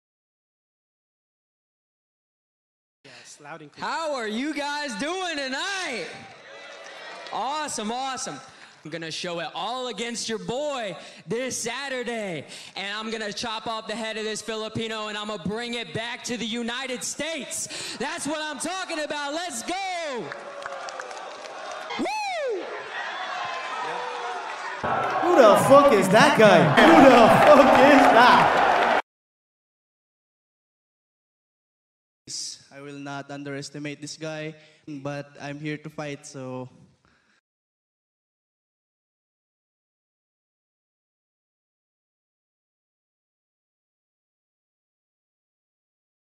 ONE FC- Jhanlo Sangiao keeps it real Jarred Brooks acting bad boy
ONE 164 Jarred Brooks disrespects Paciao but Jhanlo Sangiao keeps it real and humble in ONE 164 Presser.
Great ambassador to the sport.
Just here to fight @TeamLakay